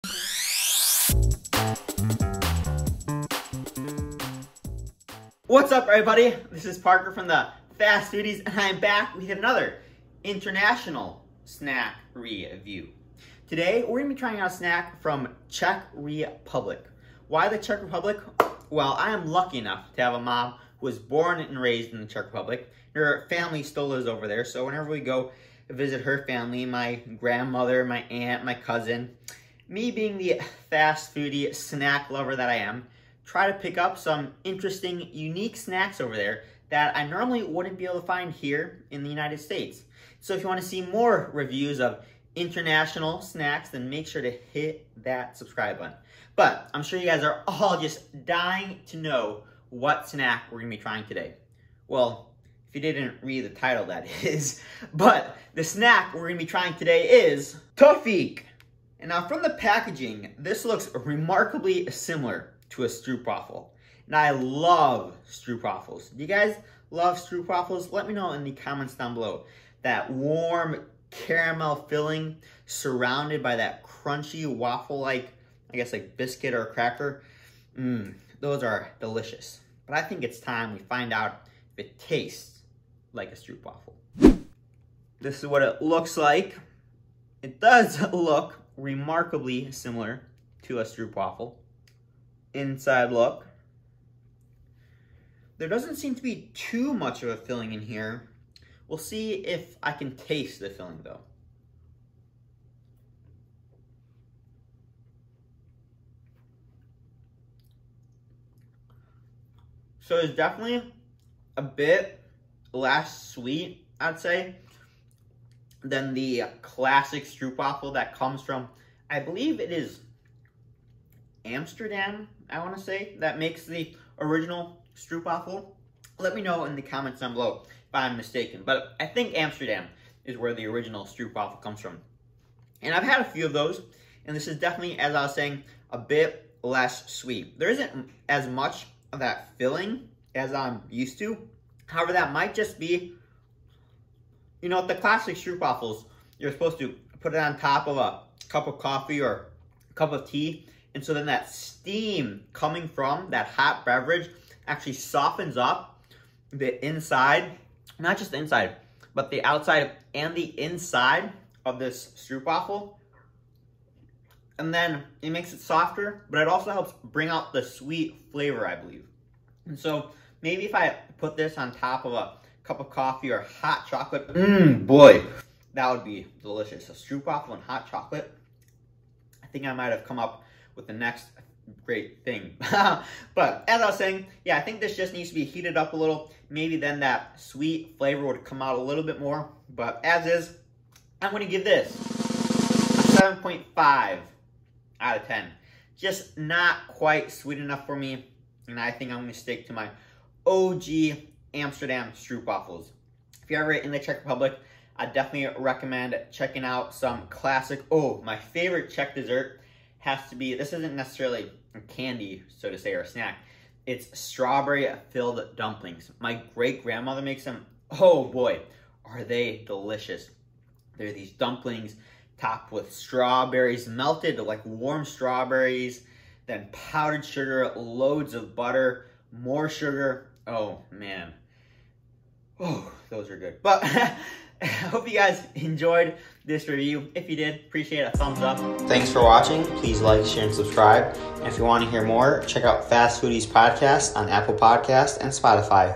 INTERNATIONAL SNACK REVIEW! Tofik from the Czech Republic! Better Than Stroopwaffles?!
Review of Tofik Candy from Czech!